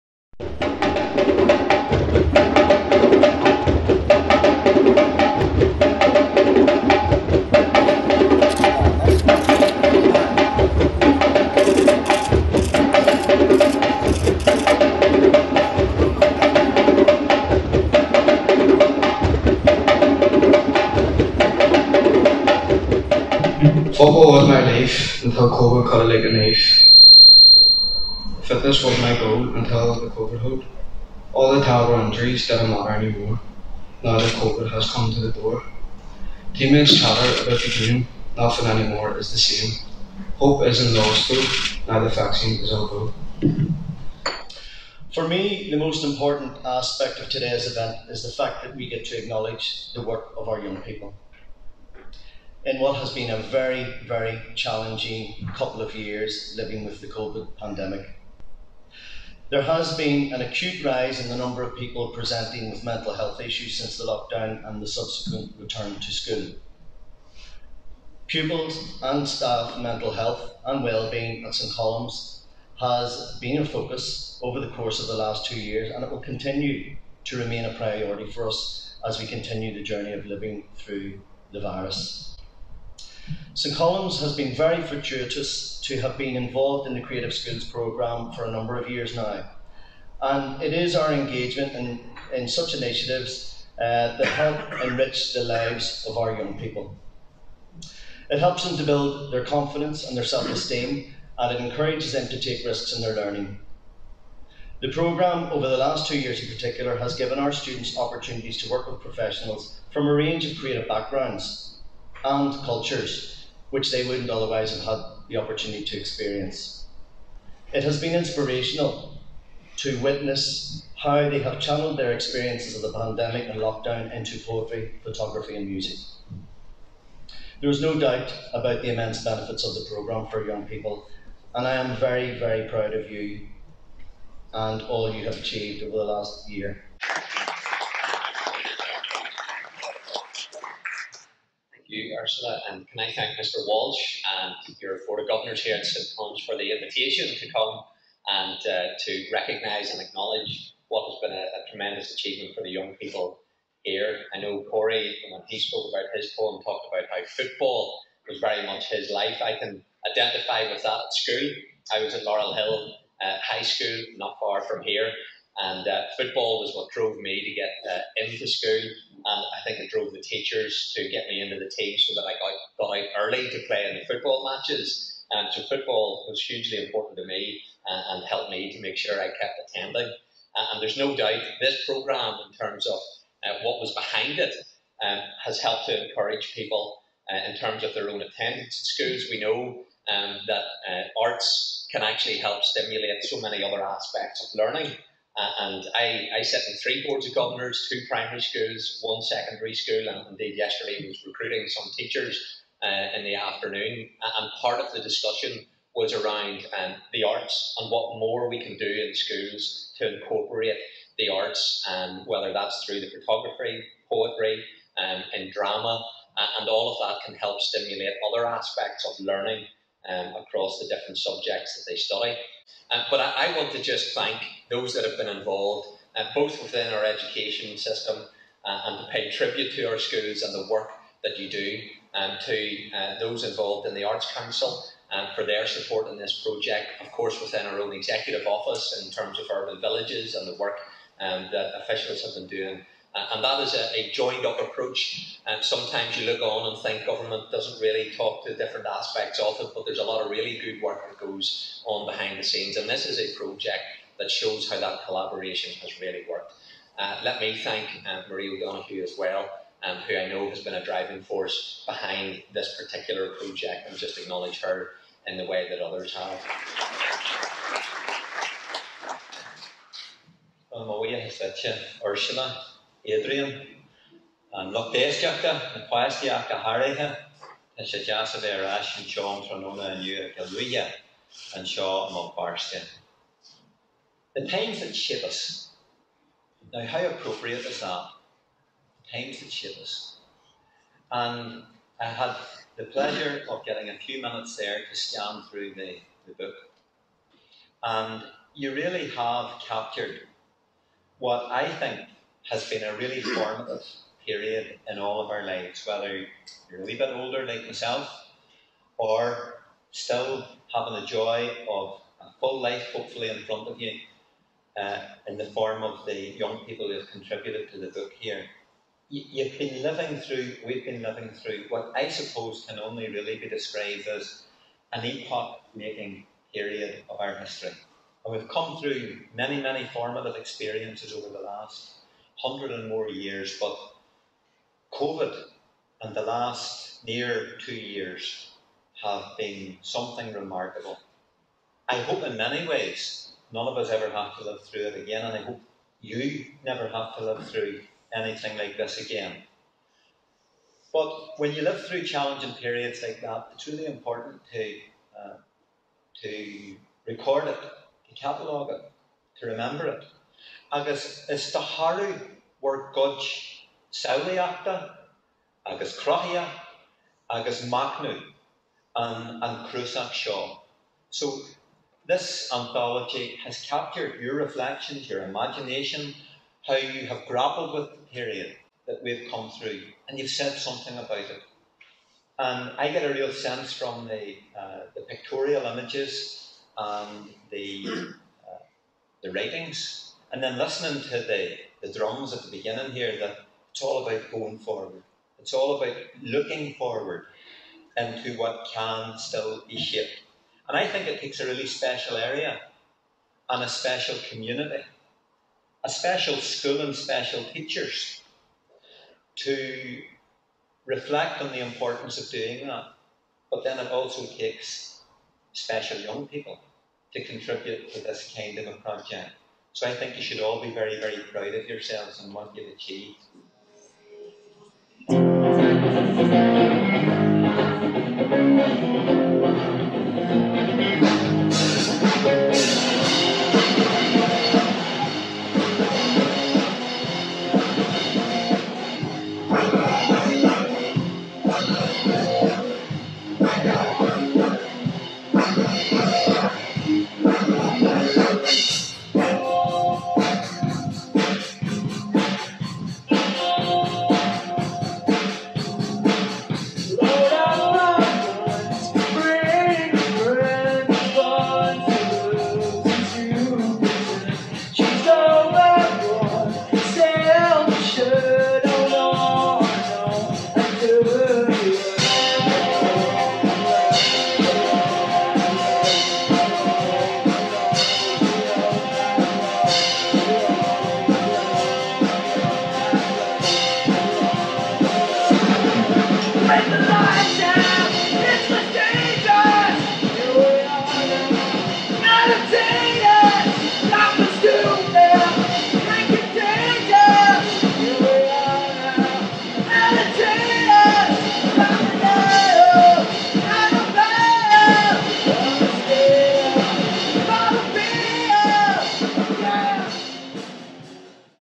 First Minister visits St Colm's High School
First Minister Paul Givan and Junior Minister Declan Kearney visit St Colm's High School in Twinbrook for the launch of a poetry anthology compiled by students.